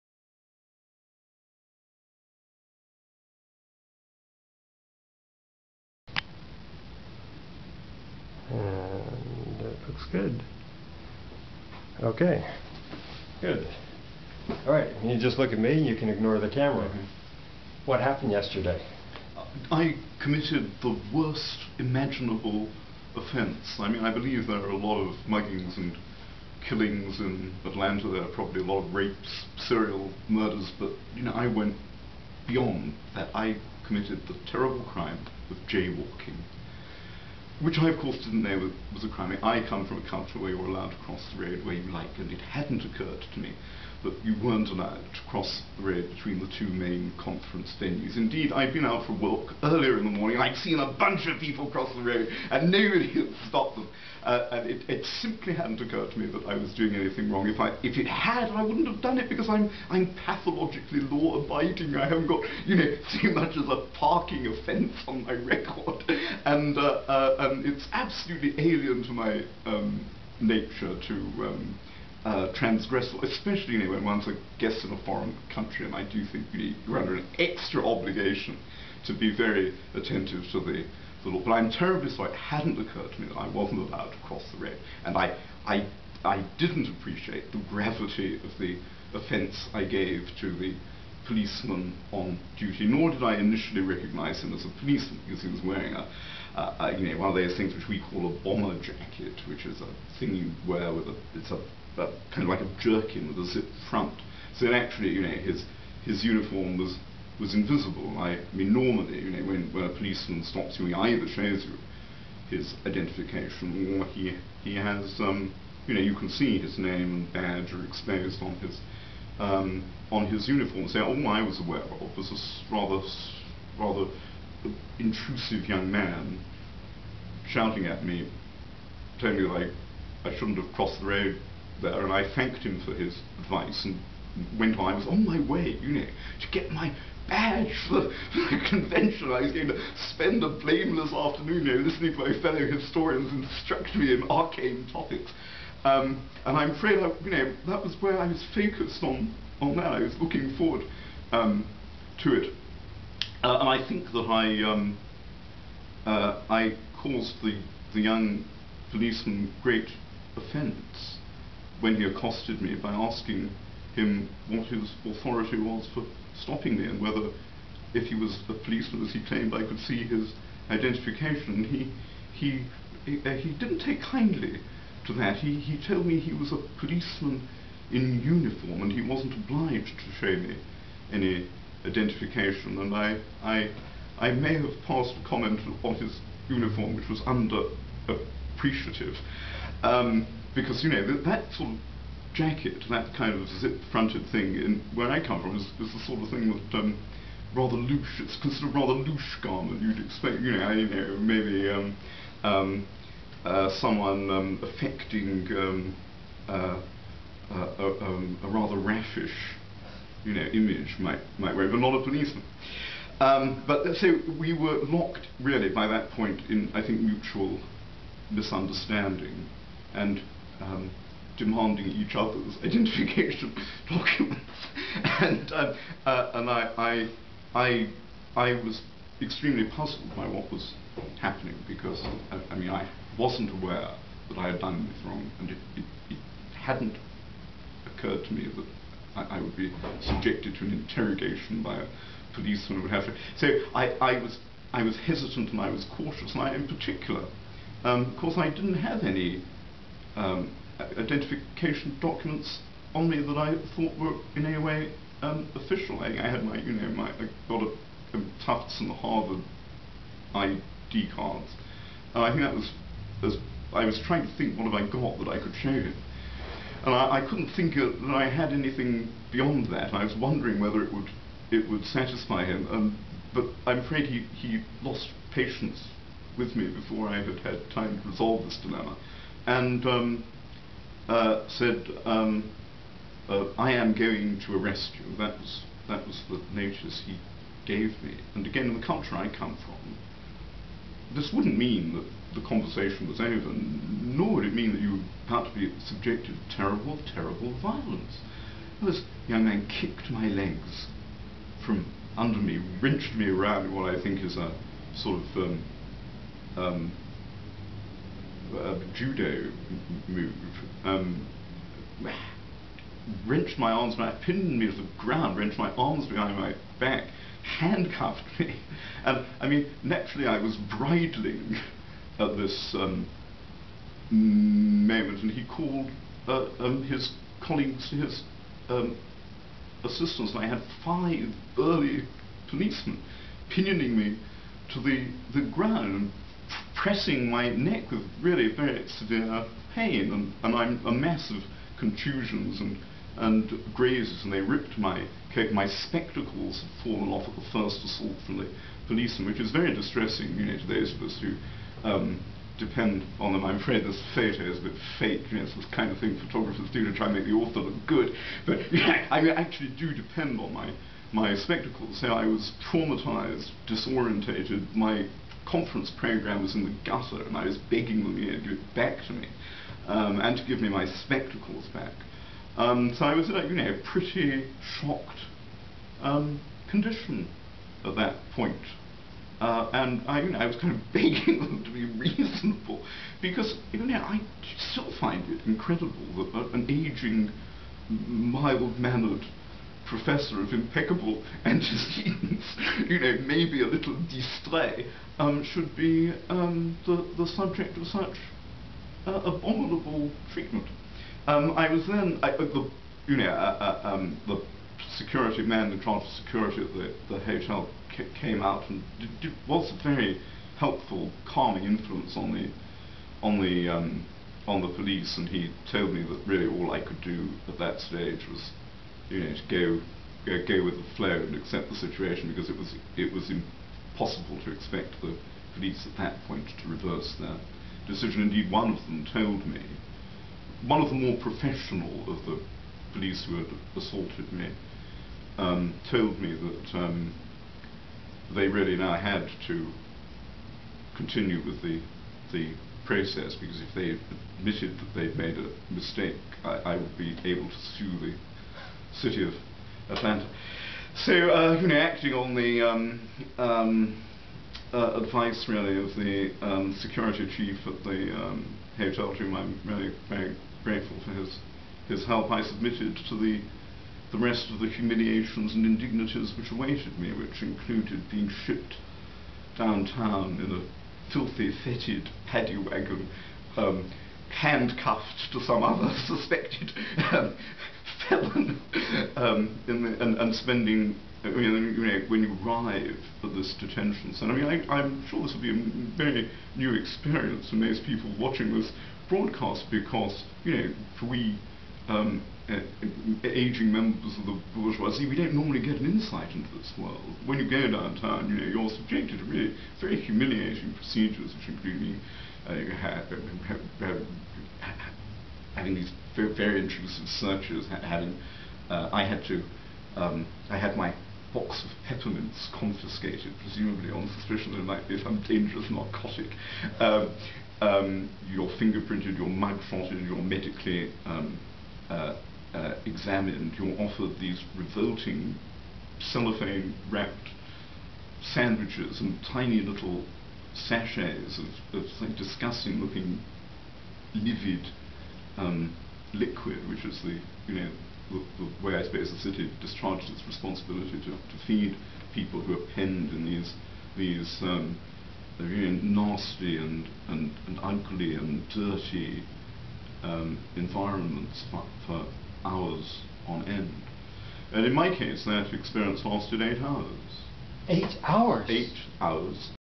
HNN: Historian's Arrest for Jaywalking
On Thursday January 4, 2007 historian Felipe Fernandez-Armesto was arrested by Atlanta police as he crossed the middle of the street between the Hilton and Hyatt hotels.  After being thrown on the ground and handcuffed, the former Oxford don and now a professor at Tufts was formally arrested and thrown in jail.  He remained incarcerated for eight hours until finally prosecutors let him go.  He was in Atlanta to attend the annual meeting of the American Historical Association.